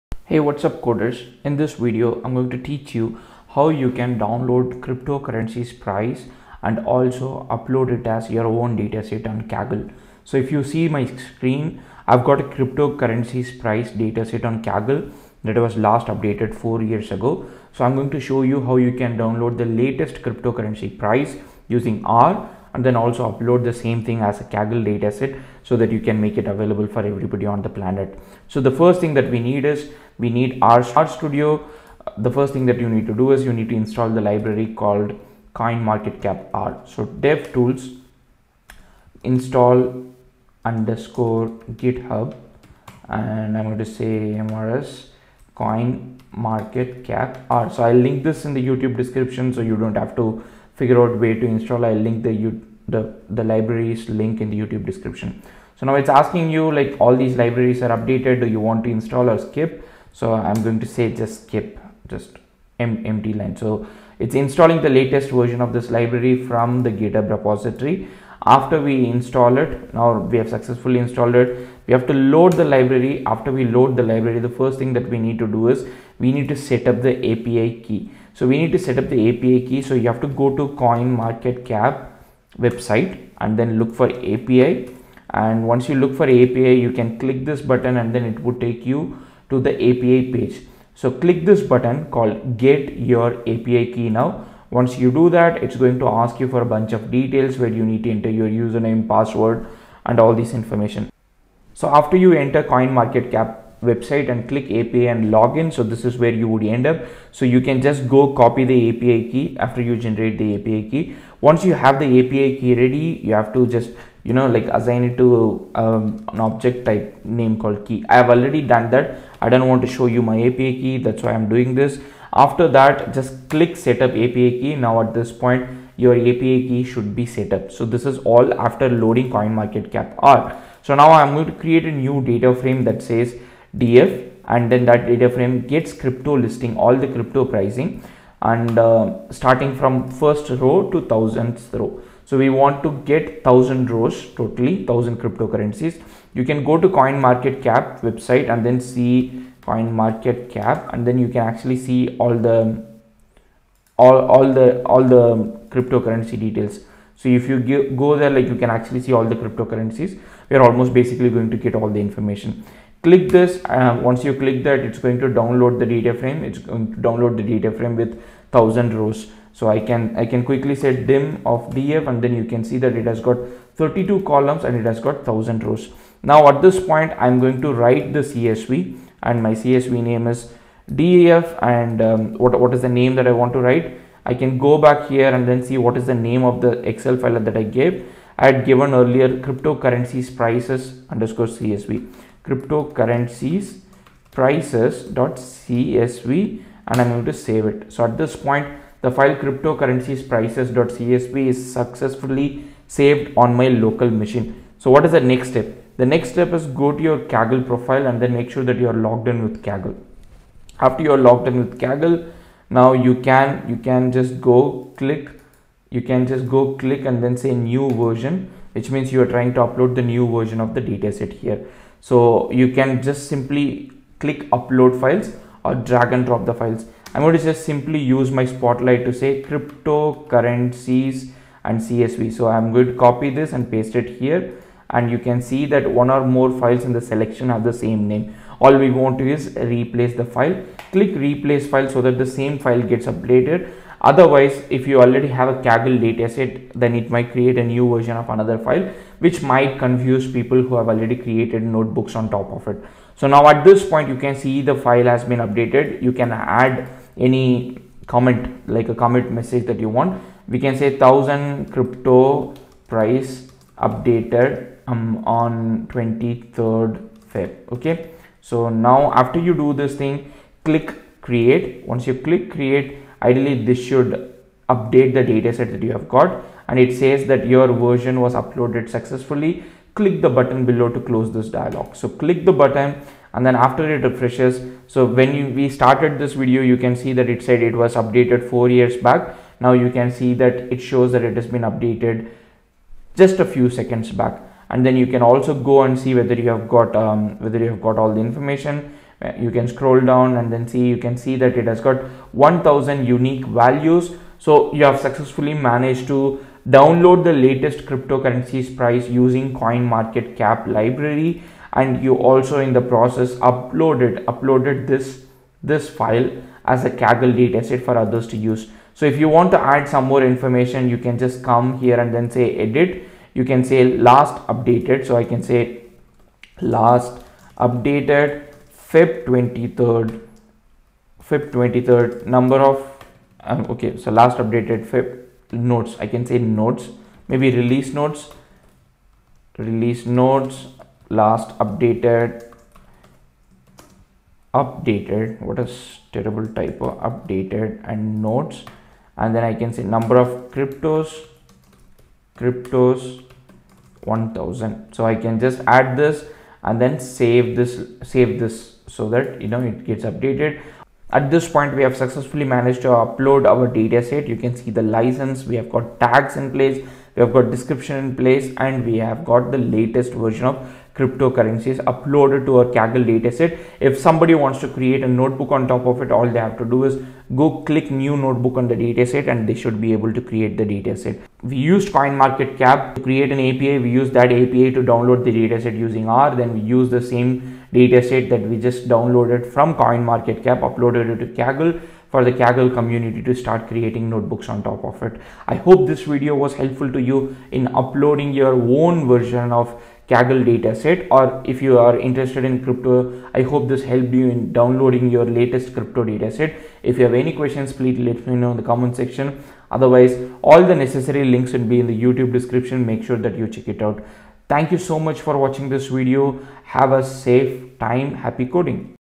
Kaggle Datasets Tutorial - Upload your Cryptocurrencies Dataset | Download Crypto data using R
1. How to download Cryptocurrencies market dataset from Coinmarketcap using R (coinmarketcapr)
2. How to get the Coinmarketcap APi
3. How to use the coinmarketcap API to download the crypto dataset
4. How to upload the crypto dataset to Kaggle datasets